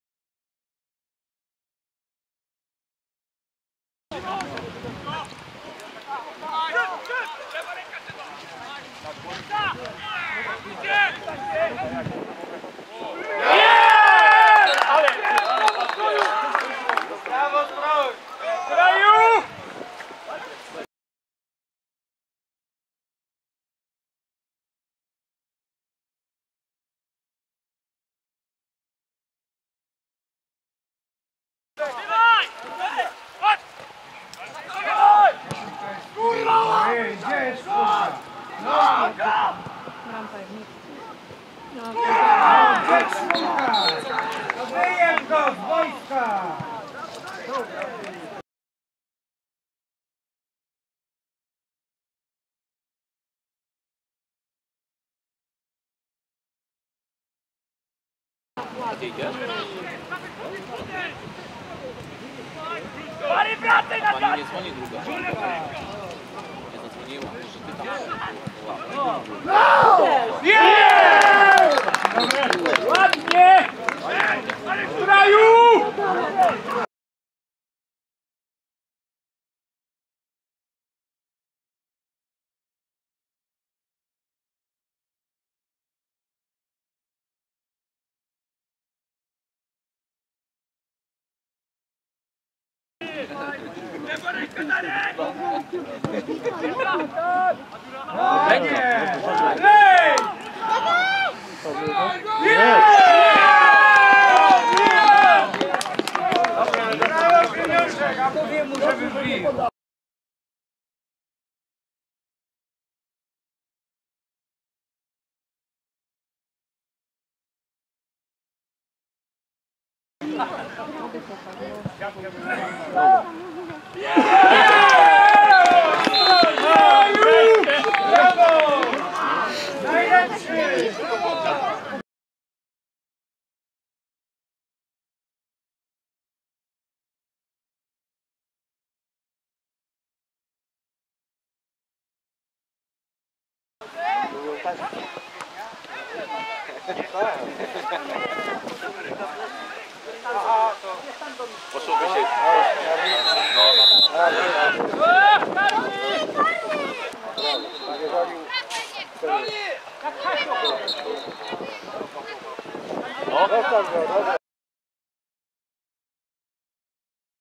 Górnik Piaski - GKS II Katowice (bramki)
XIV kolejka IV ligi śląskiej gr. I
Górnik Piaski - GKS II Katowice 4:2 (2:1)
5' - Marcin Strojek
8' - Eryk Sobków
16' - Kamil Szydłowski
54' - Marcin Pieniążek
58' - Marcin Strojek
79' - Sebastian Szombierski (rzut karny)

Jeśli spodobał Ci się nasz kanał prosimy o subskrypcję a na pewno nie ominie Cię nic co się na nim pojawi.

Nie zapomnij zostawić "łapkę" w górę !